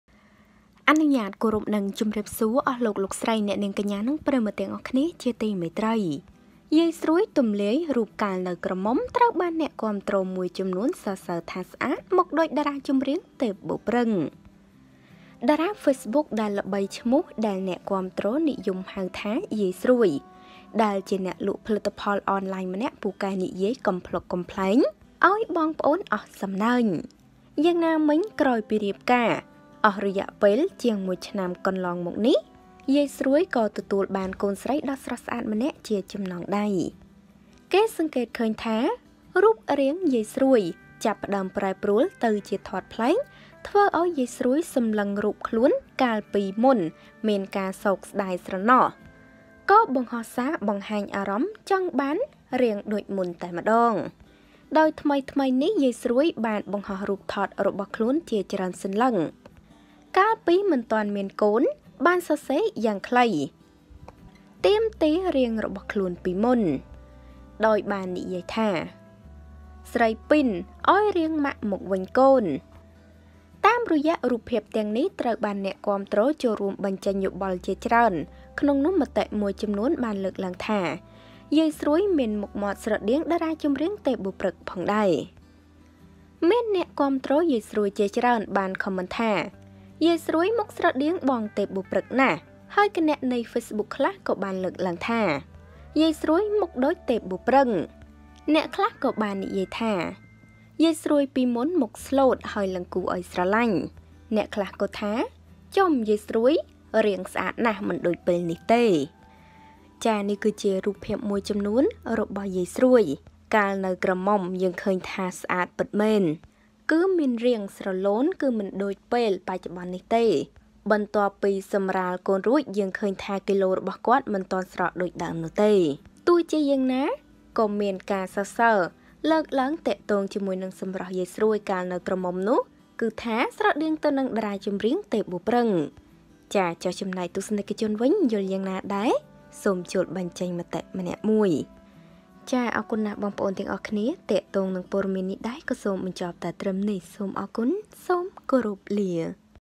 នោះ!យាយ​សួយ ទម្លាយរូបកាលនៅក្រមុំ ត្រូវ​បាន​អ្នកគាំទ្រ​​សរសើរថាស្អាត មុខ​ដូច តារាចម្រៀងទេព បូ​ព្រឹក្ស
ថ្មីៗនេះ យាយ សួស បាន​បង្ហោះ​រូបថត​របស់​ខ្លួន​ជាច្រើន​សន្លឹក កាល​មិនទាន់​មានកូន បាន​សរសេរ​យ៉ាងខ្លី ទាមទារ​រាង​របស់ខ្លួន​ពីមុន ​ថា «​ស្រី​ពី​ន​អោយ​រាង​ម៉ាក់​វិញ​កូន​»​។​​​​​​​​​​​​​​​​​​​​​​​​​​​​​​​​​​​​​​​​​​​​​​​​​​​​​​​​​​​​​​​​​​​​​​​​​​​​​​​​​​​​​​​​​​​​​​​​​​​​​​​​​​​​​​​​​​​​​​​​​​​​​​​​​​​​​​​​​​​​​​​​​​​​​​​​​​​​​​​​​​​​​​​​​​​​​​​​​​​​​​​​​​​​​​​​​​​​​​​​​​​​​​ តាមរយៈ​រូបភាព​ទាំងនេះ ត្រូវបាន​អ្នកគាំទ្រ ចូលរួម​បញ្ចេញយោបល់​ជាច្រើន ក្នុងនោះ​មតិ​មួយចំនួន​បានលើកឡើង​ថា យាយ​ស្រួយ​មានមុខមាត់​ស្រដៀង​តារាចម្រៀង​ទេព បូ​ព្រឹក្ស​ផងដែរ​។​

សូមអធ្យាស្រ័យ

ចំពោះការមិនបានតប ខំមិនបានគ្រប់គ្នា

Ms Socheata